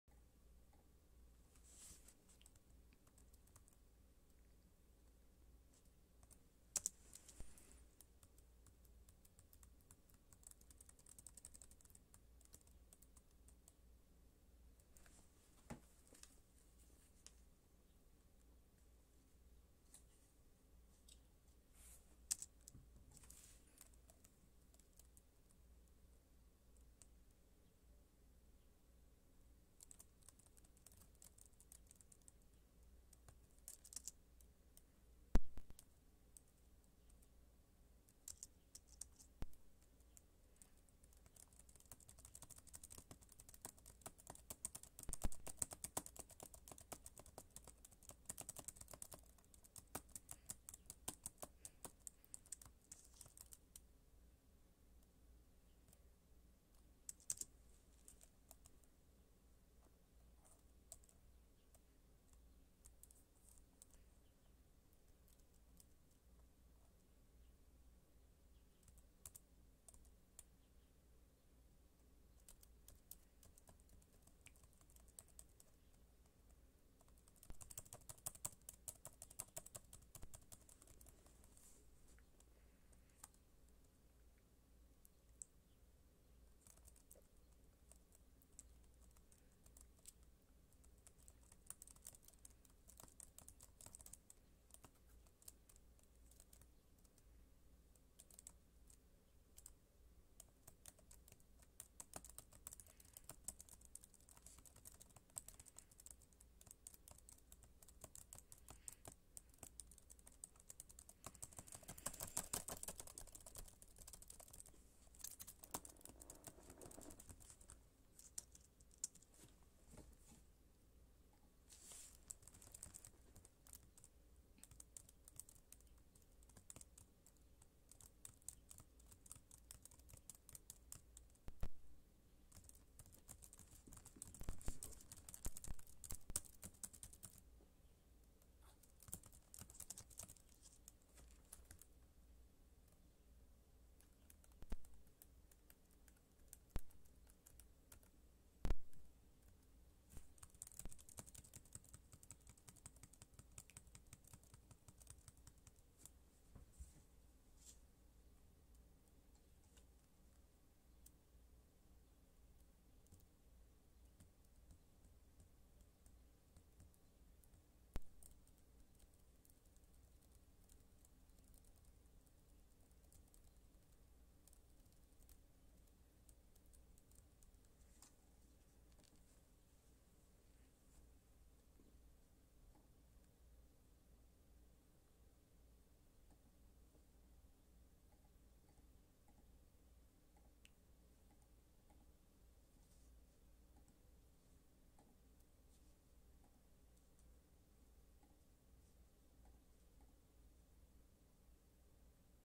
I HATE THIS GAME(WARNING EXTREMELY BORING)
I wanted to go back to 2015 and play this game so I did and IT WA THE HARDEST THING I EVER PLAYED